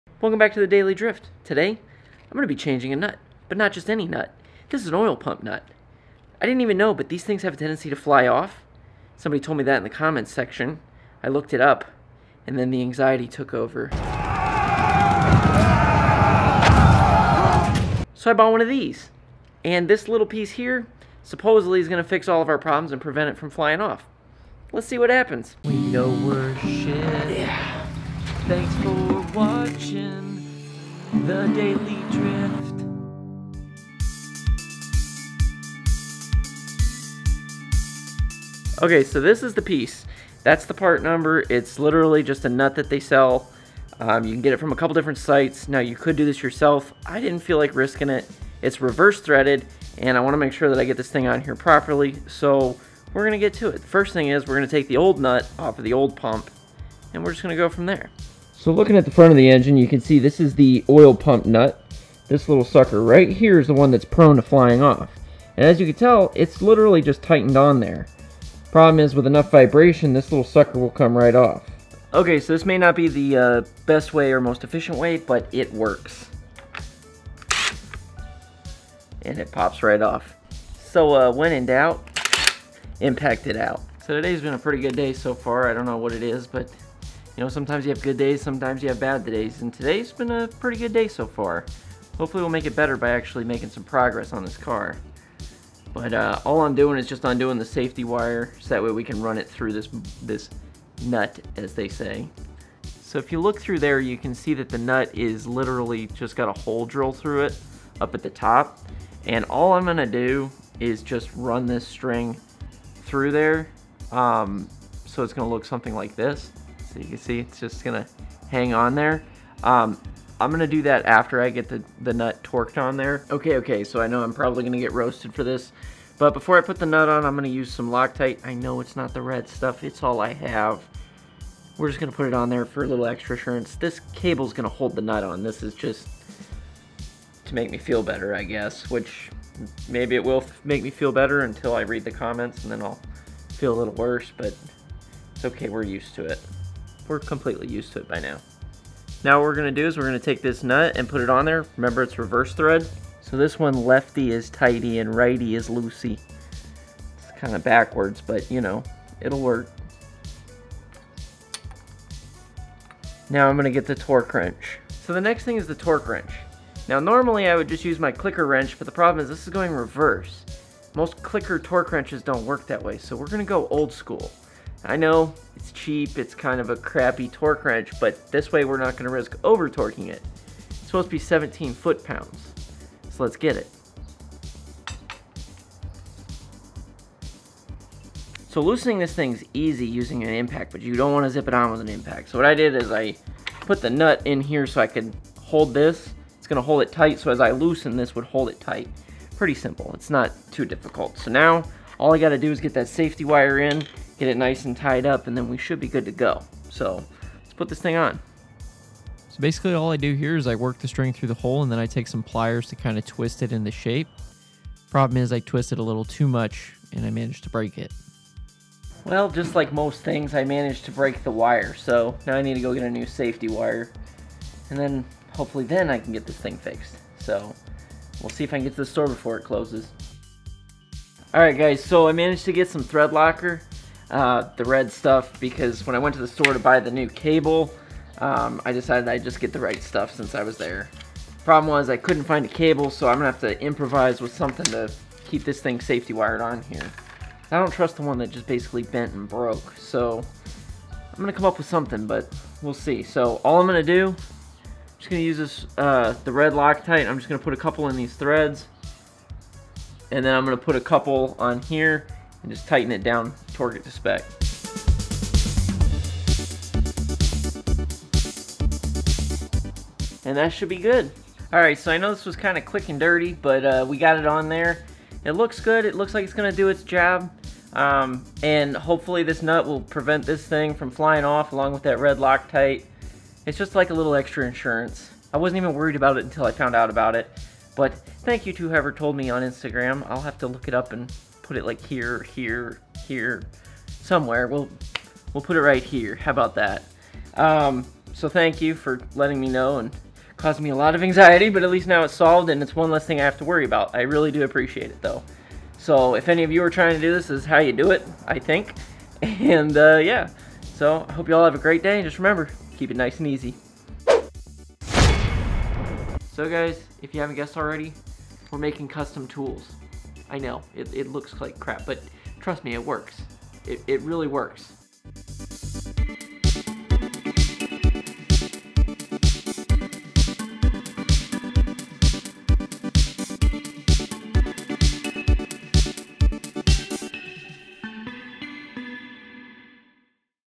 BMW E36 Oil Pump Nut Mod
Today I am going to fix a common E36 problem on the Dubya. Evidently the Oil Pump Nut can come loose with a lot of excess vibration (Clutch Kicking, Drifting, Lighter Flywheel, Etc), you know Daily Drifter stuff 😂.

So since we have access to the oil pump, we may as well do this little upgrade to potentially save our engine. The fix is to add some red Loctite and drill a very tiny hole through the oil pump nut so you can secure it with safety wire. This is commonly done on Airplanes to secure nuts and bolts that are prone to high vibrations. If it is good enough for a plane it will be good enough for the Dubya.
Social Garage:
-Mint Chocolate 2a